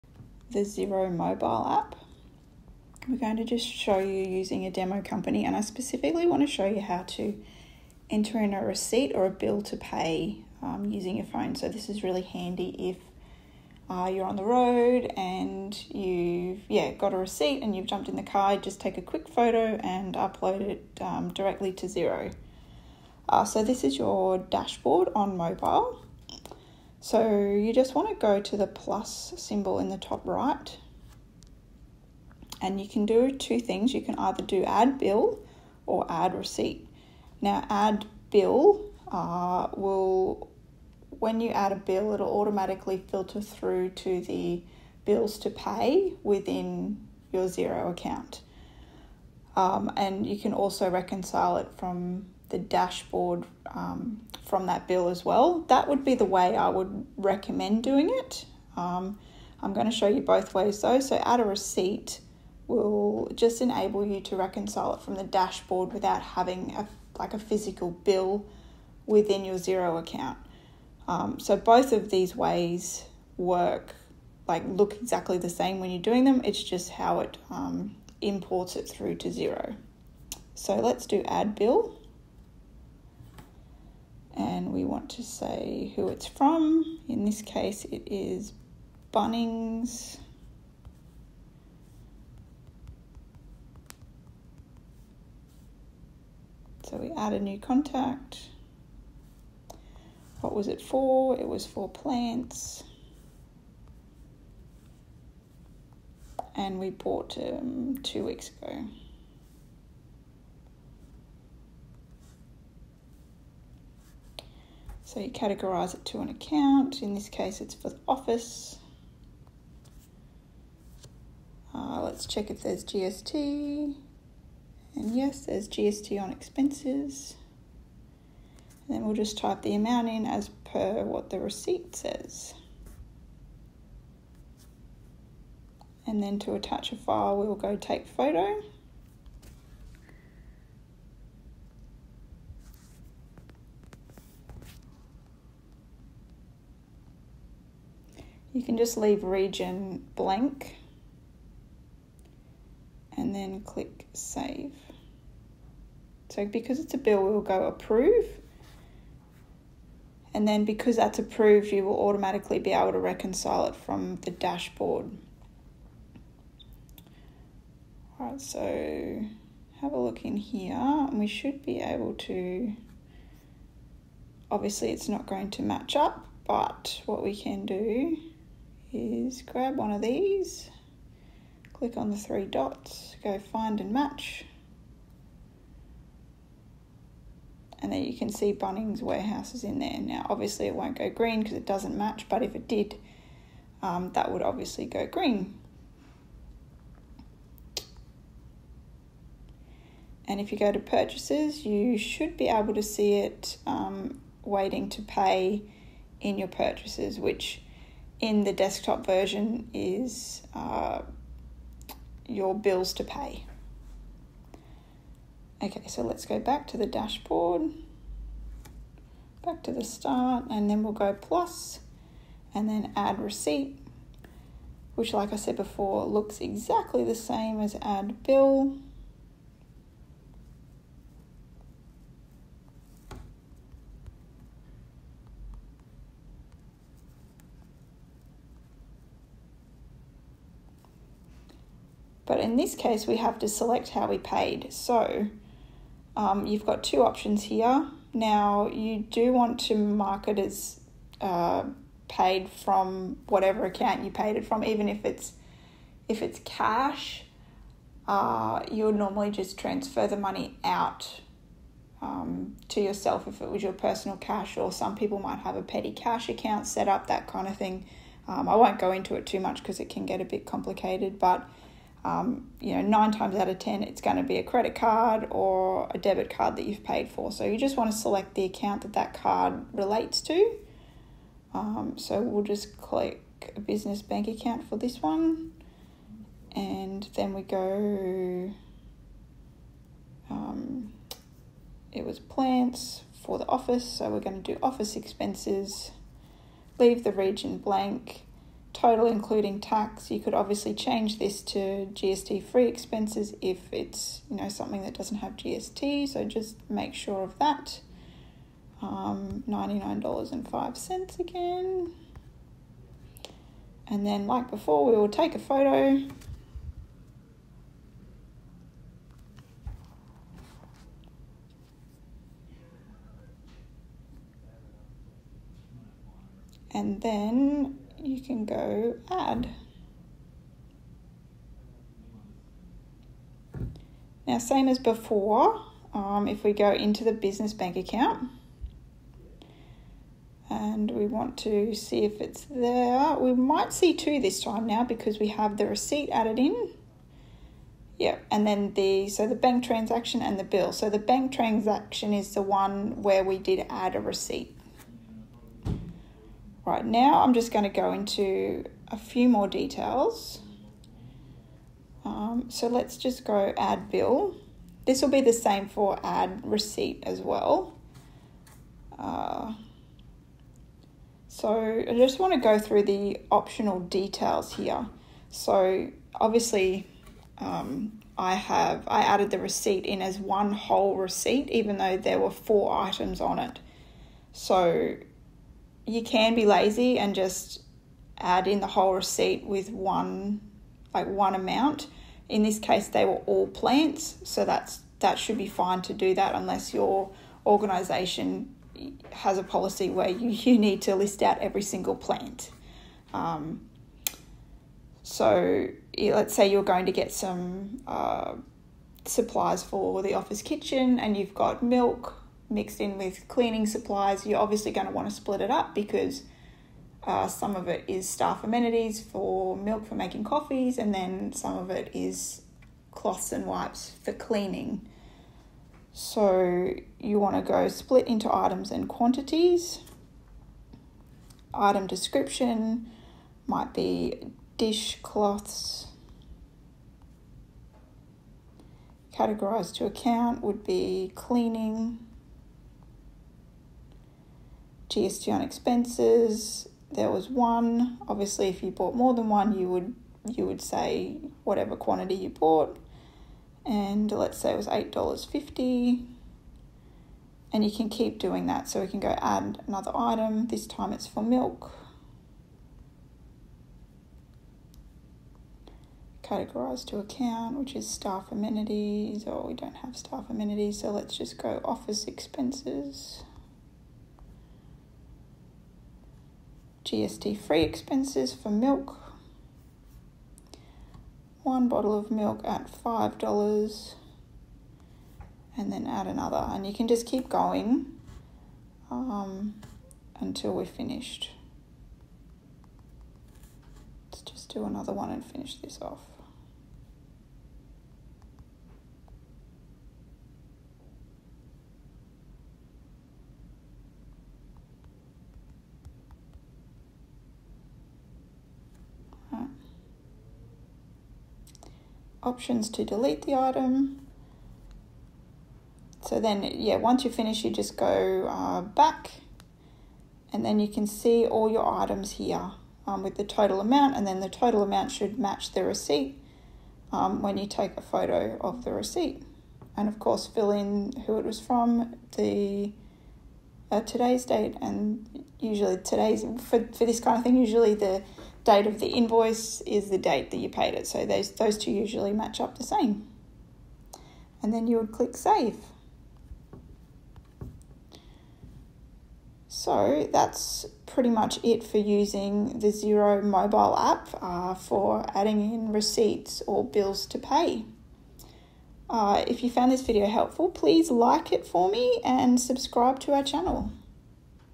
How to add receipts into Xero using the Xero mobile app.
In this video I specifically show you how to enter in a receipt using the xero mobile app. I run through how to add a receipt using the add bill and add receipt functions. I then explain the difference between these two features. At the end of the video I explain how to add in multiple lines of a receipt rather than just one whole amount, so watch until the end.